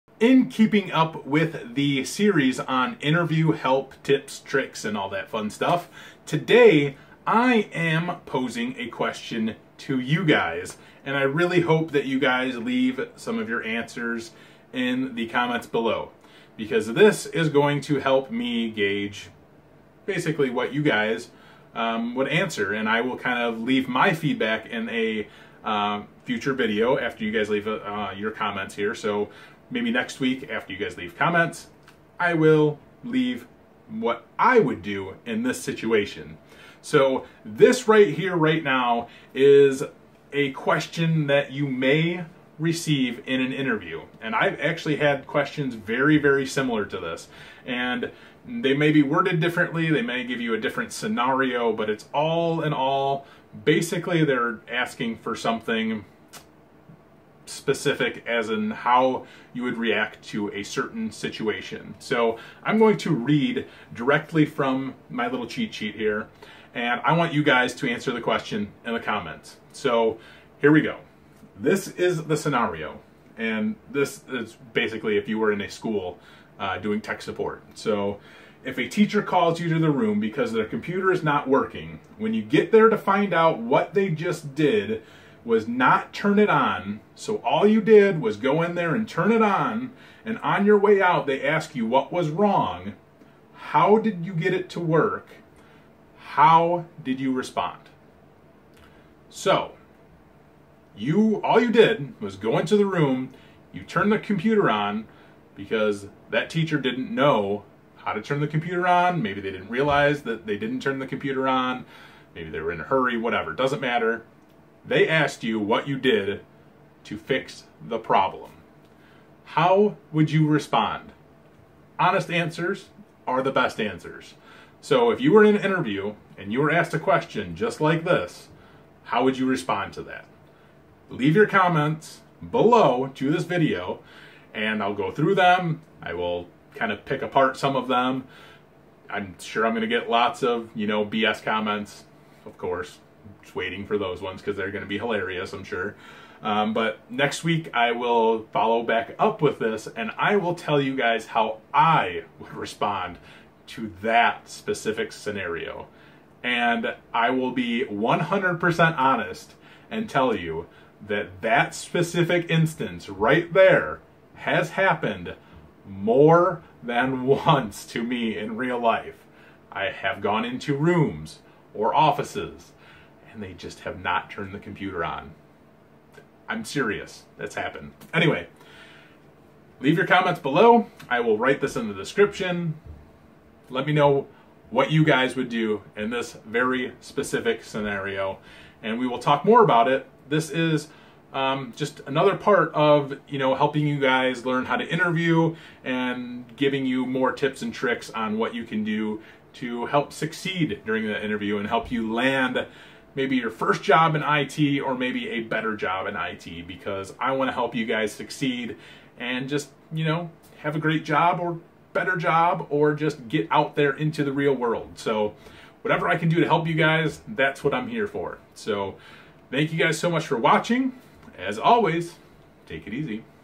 Answer this I.T. Interview Question to See How You Stack Up...
This week I am asking you guys to answer a question for me.  I'd like to keep going with the Interview Help Series by taking an actual question from an interview questioner and asking you guys to answer it for me.  Here's the question:

If a teacher calls you to their room because their computer is not working. When you get there to find out that they just did not turn it on so you turn it on. On your way out they ask what was wrong, how did you get it to work. How would you respond.

You would be amazed at how many people I have been able to weed out with this one. Anywhere from rude answers, to ones that would make a teacher feel dumb. As a tech person this is probably one of the hardest situations they could face when it comes to interpersonal communications.

Thank you for visiting my YouTube channel.  Please find see below for more places to find me: